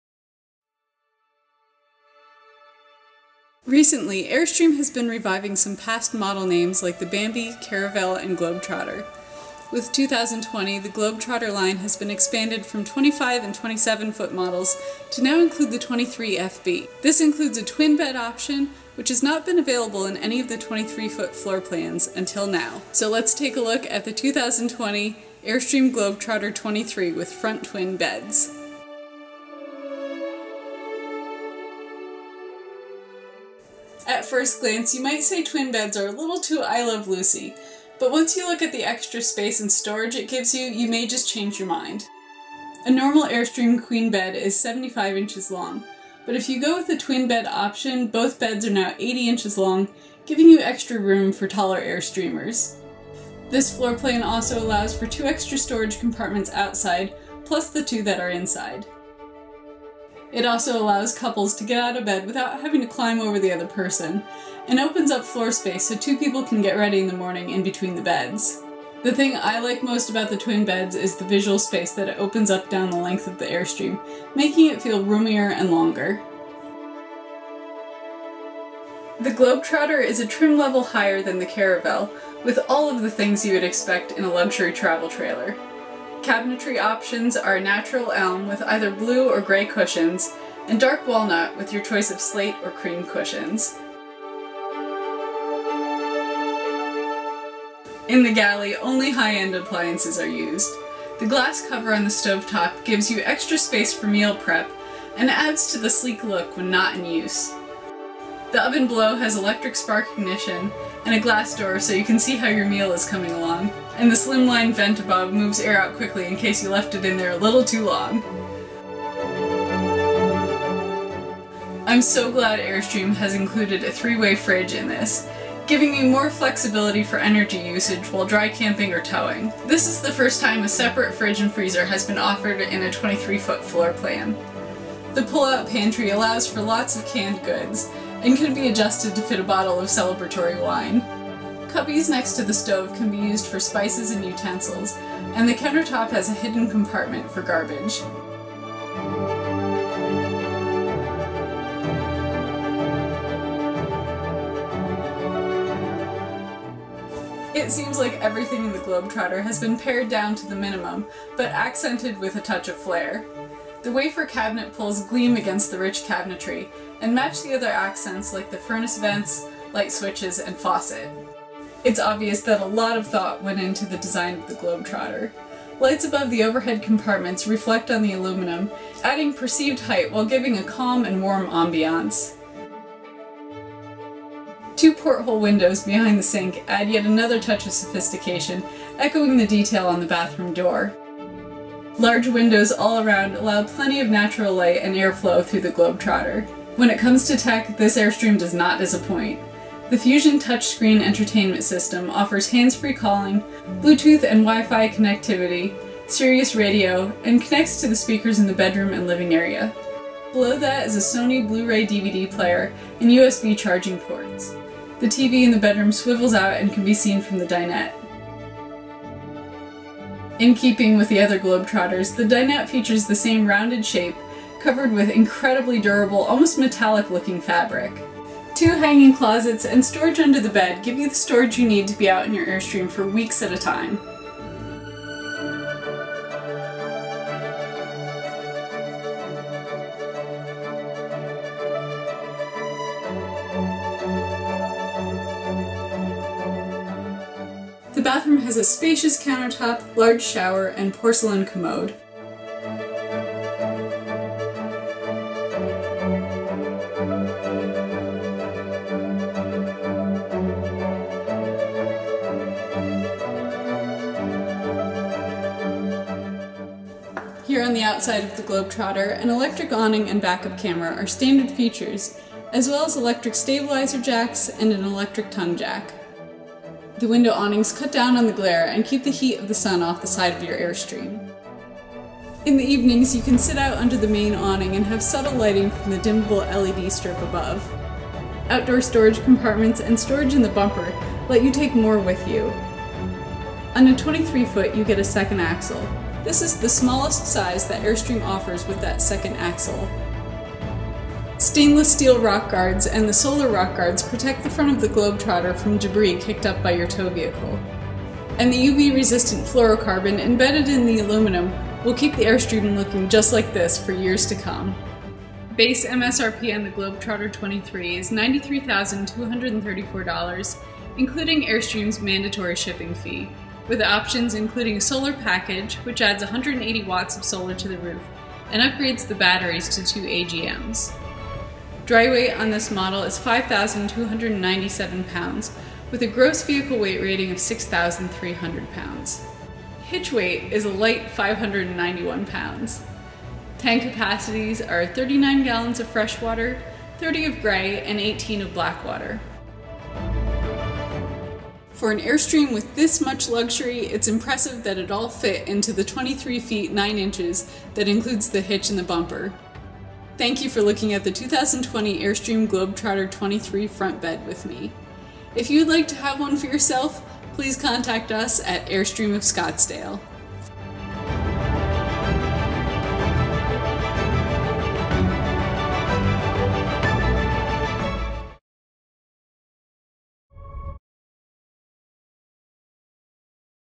2020 Globetrotter 23FB with Twin Beds
The 2020 Globetrotter 23 brings a few firsts for Airstream, like offering twin beds and separate fridge and freezer compartments in a length shorter than 25 feet. Deep, rich toned cabinetry or a lighter more airy option are available in the Globetrotter, but one thing that is not optional is luxury!